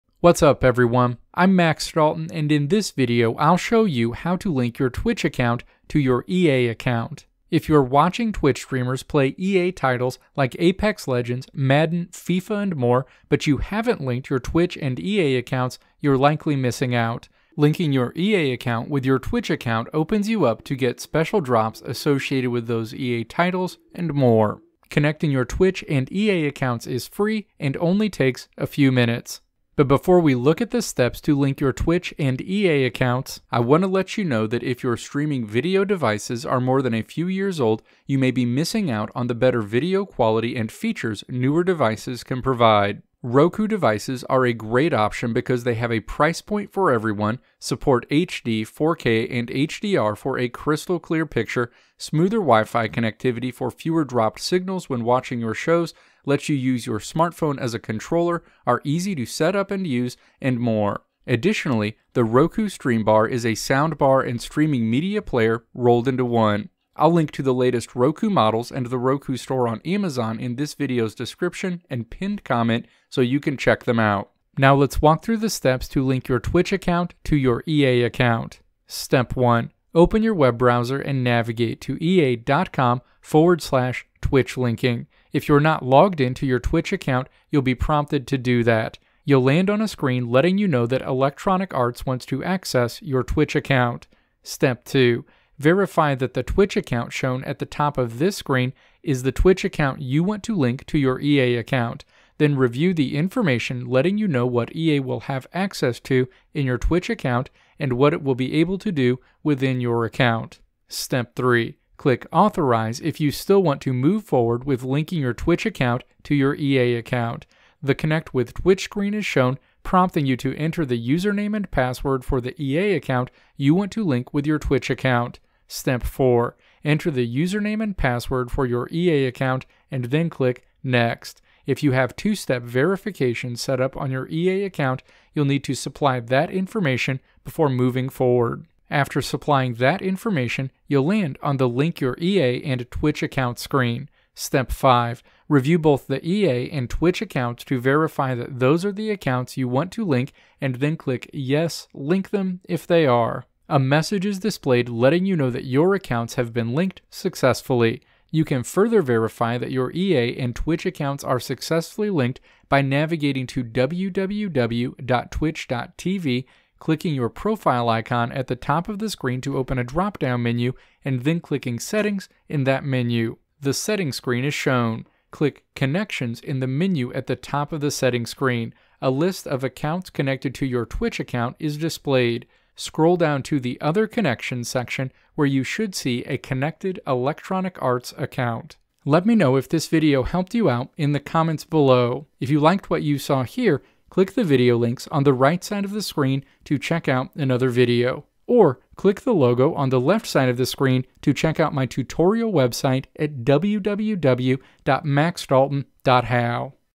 How to Link Twitch to EA Account
In this video I'll show you how to link your Twitch account to your EA account.

Check out the latest from Roku!
Roku Express 4K+
Roku Streaming Stick+
Roku Ultra
Roku Streambar
Roku Store

Timestamps:
Introduction: 0:00
Steps to Link Twitch to EA Account: 1:14
Conclusion: 3:09

Video Transcript:
If you're watching Twitch streamers play EA titles, like Apex Legends, Madden, FIFA and more, but you haven't linked your Twitch and EA accounts, you're likely missing out. Linking your EA account with your Twitch account opens you up to get special drops associated with those titles. Connecting your Twitch and EA accounts is free, and only takes a few minutes.

Now let's walk through the steps to link your Twitch account to your EA account.

Step 1. Open your web browser and navigate to ea.com/twitchlinking. If you're not logged into your Twitch account you'll be prompted to do that. You'll land on a screen letting you know that Electronic Arts wants to access your Twitch account.

Step 2. Verify that the Twitch account shown at the top of this screen is the Twitch account you want to link to your EA account, then review the information letting you know what EA will have access to in your Twitch account, and what it will be able to do within your account.

Step 3. Click "Authorize" if you still want to move forward with linking your Twitch account to your EA account. The Connect With Twitch screen is shown, prompting you to enter the username and password for the EA account you want to link with your Twitch account.

Step 4. Enter the username and password for your EA account, and then click "Next." If you have two-step verification set up on your EA account, you'll need to supply that information before moving forward. After supplying that information you'll land on the Link Your EA and Twitch Accounts screen.

Step 5. Review both the EA and Twitch accounts to verify that those are the accounts you want to link, and then click "Yes, Link Them" if they are. A message is displayed letting you know that your accounts have been linked successfully.

You can further verify that you're EA and Twitch accounts are successfully link by navigating clicking your profile icon at the top of the screen to open a drop-down menu, and then clicking "Settings" in that menu. The Settings screen is shown. Click "Connections" in the menu at the top of the Settings screen. A list of account connected to your Twitch account is displayed. Scroll down to the Other Connections section, where you should see a connected Electronic Arts account.